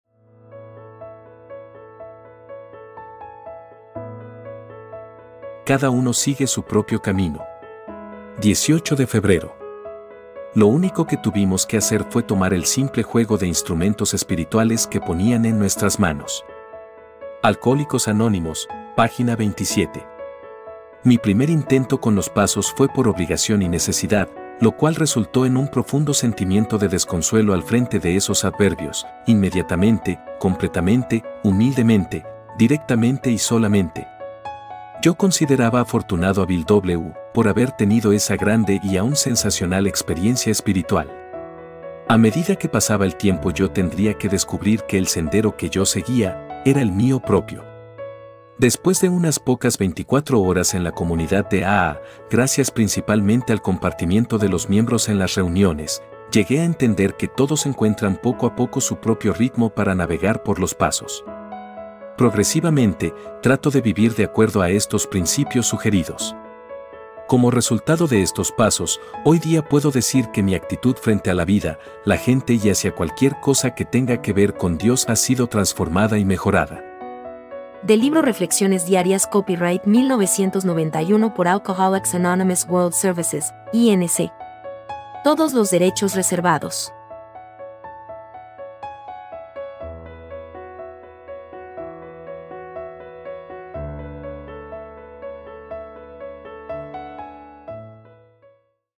CADA UNO SIGUE SU PROPIO CAMINO - Febrero 18
…lo único que tuvimos que hacer fue tomar el simple juego de instrumentos espirituales que ponían en nuestras manos.
ALCOHÓLICOS ANÓNIMOS, pág. 27
Mi primer intento con los pasos fue por obligación y necesidad, lo cual resultó en un profundo sentimiento de desconsuelo al frente de esos adverbios: inmediatamente, completamente, humildemente, directamente y solamente. Yo consideraba afortunado a Bill W. por haber tenido esa grande y aun sensacional experiencia espiritual. A medida que pasaba el tiempo yo tendría que descubrir que el sendero que yo seguía, era el mío propio. Después de unas pocas veinticuatro horas en la comunidad de aa, gracias principalmente al compartimiento de los miembros en las reuniones, llegué a entender que todos encuentran poco a poco su propio ritmo para navegar por los pasos. Progresivamente, trato de vivir de acuerdo a estos principios sugeridos. Como resultado de estos pasos, hoy día puedo decir que mi actitud frente a la vida, la gente y hacia cualquier cosa que tenga que ver con Dios ha sido transformada y mejorada.